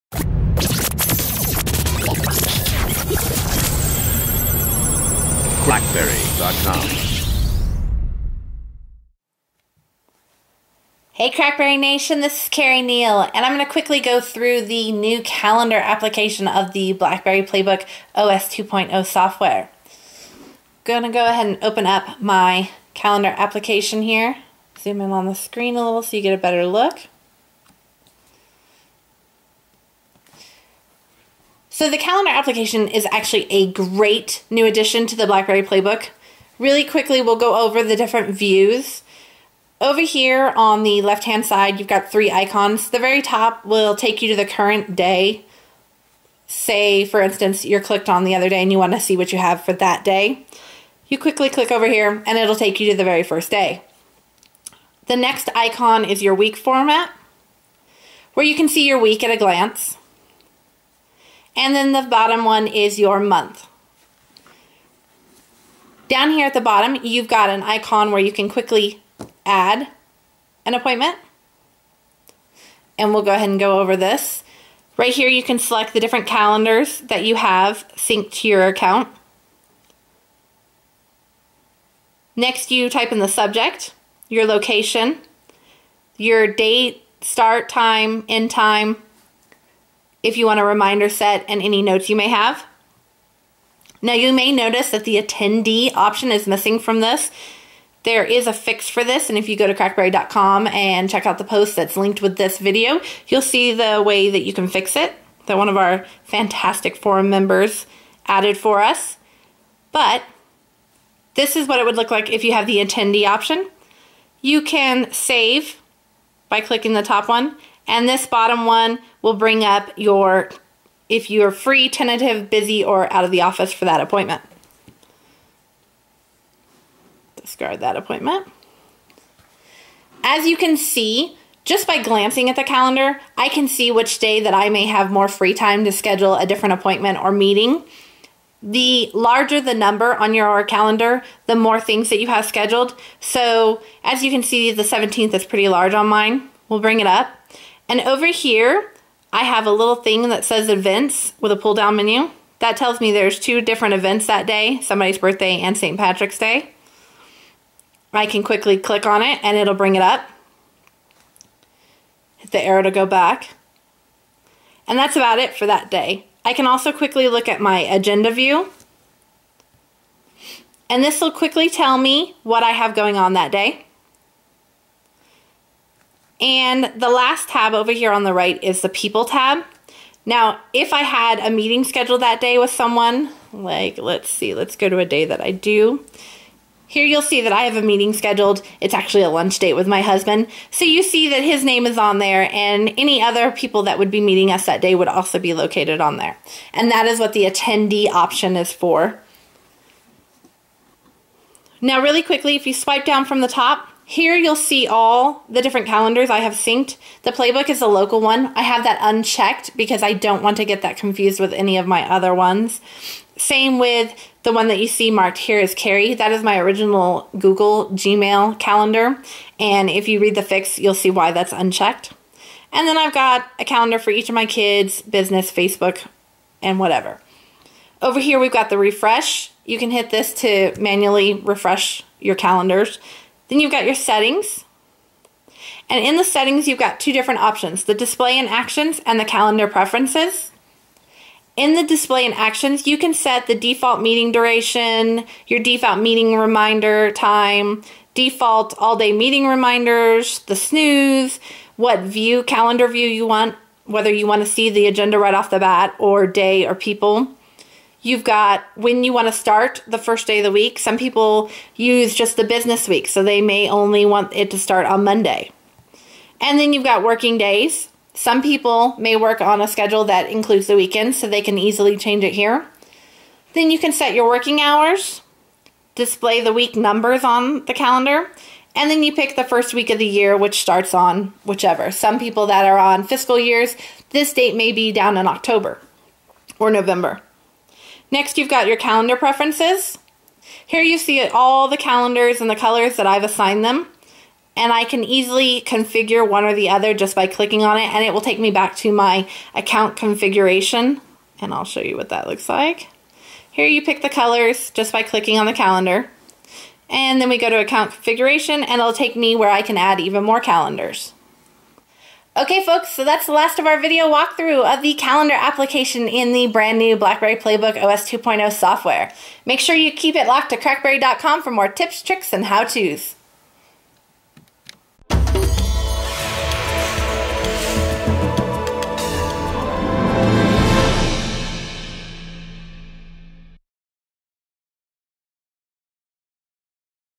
BlackBerry PlayBook Calendar Walkthrough
A look at the Calendar app on the BlackBerry PlayBook